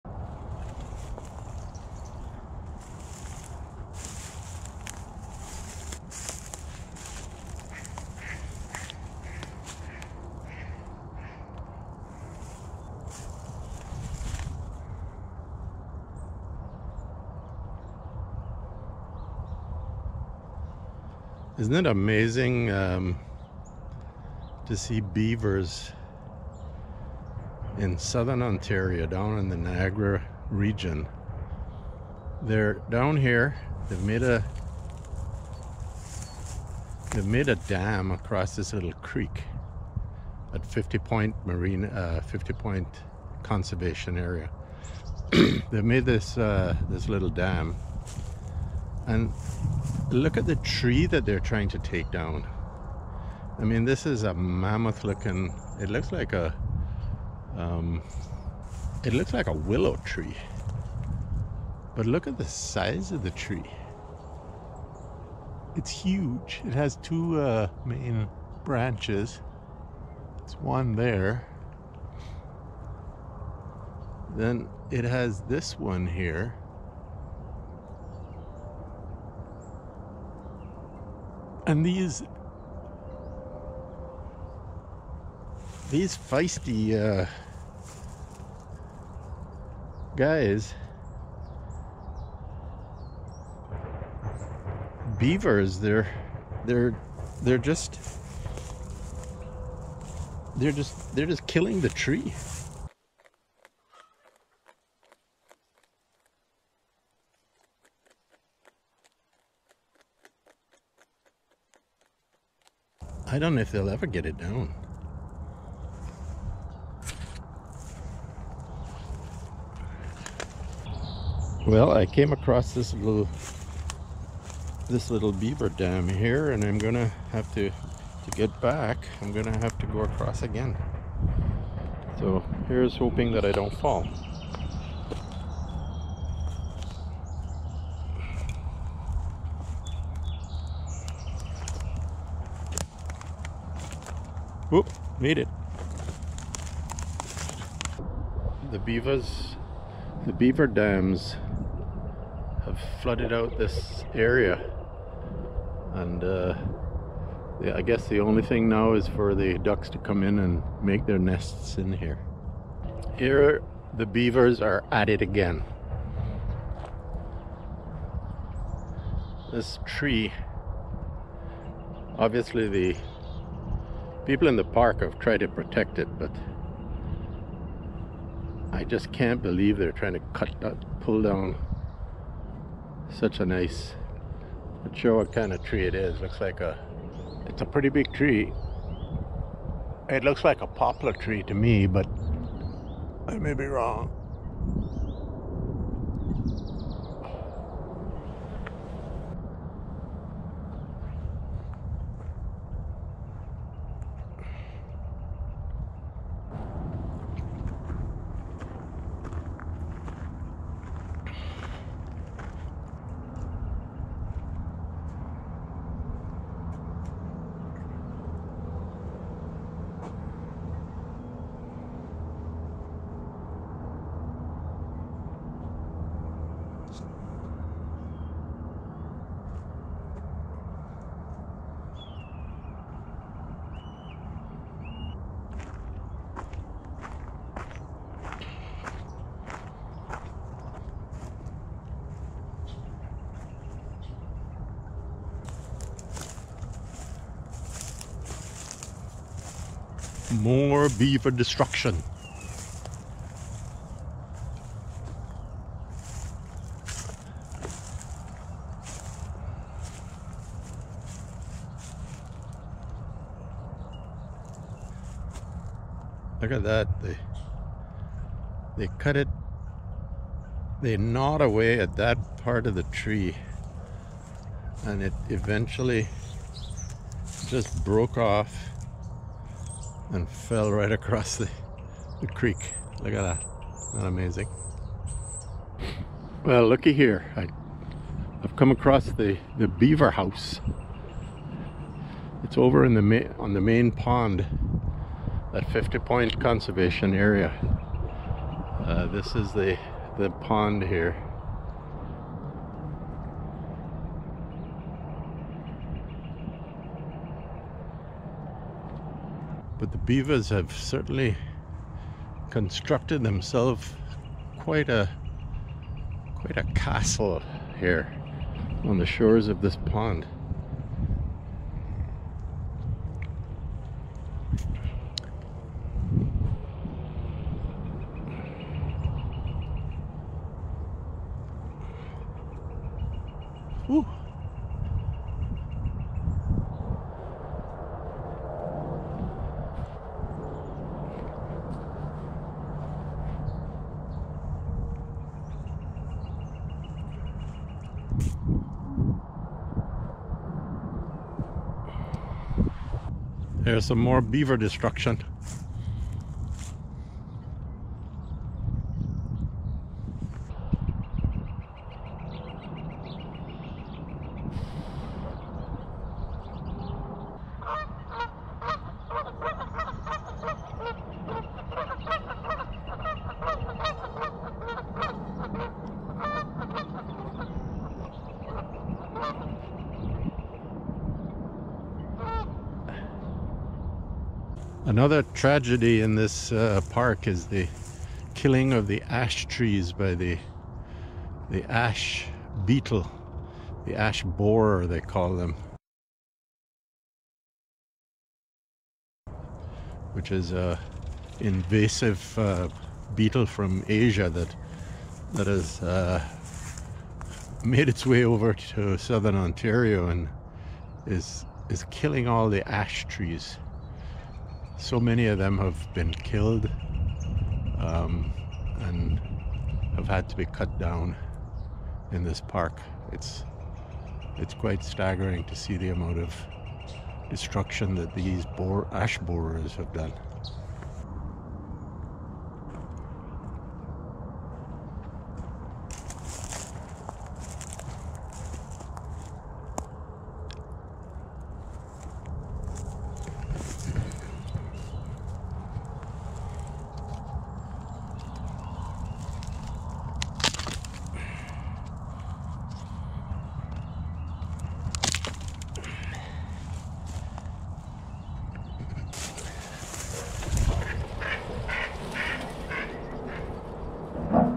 Beaver's cutting down trees.
Walking through Fifty Point Conservation forest in the Hamilton Niagara area this spring, one cannot help but notice how nature has adversely affected the tree population in the park. Beavers on one hand have indiscriminately chewed away at some pretty large trees in their quest for survival in this fairly small ecosystem. On the other hand, the Emerald Ash Borer Beetle has killed many ash trees as well. Fortunately the devastation by the Beaver is limited to the park alone, whereas the ash trees have been decimated by this invasive species at an enormous rate in all of Ontario Canada & I might add in all of North America. I thought I would publish this video to show how nature affects our environment.